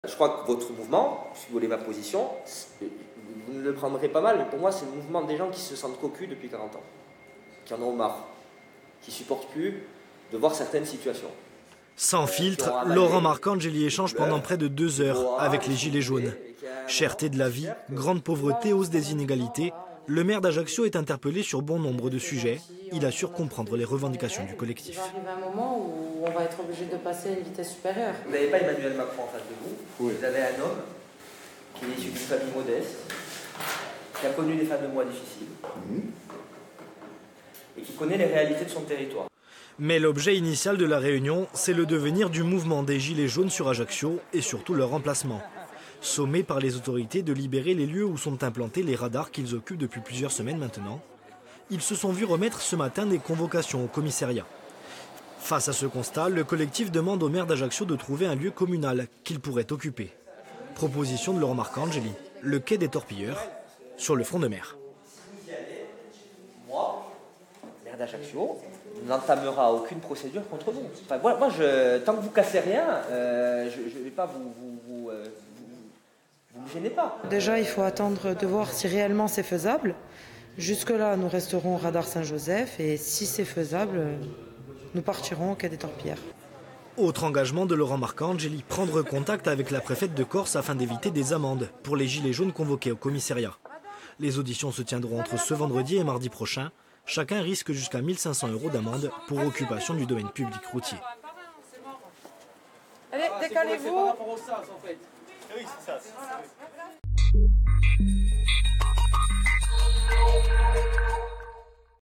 Ajaccio : Les Gilets Jaunes convoqués au commissariat
A Ajaccio les Gilets Jaunes sont priés de libérer les radars. Ce vendredi les autorités leur ont remis des convocations au commissariat d'Ajaccio pour occupation du domaine publique. Ils ont rencontré le maire d'Ajaccio Laurent Marcangeli dans l'après-midi.

Intervenants -Laurent Marcangeli, maire d'Ajaccio // Noëlle Andarelli, porte-Parole Gilets Jaunes Ajaccio

Equipe - Pierre Simompoli / Franck Rombaldi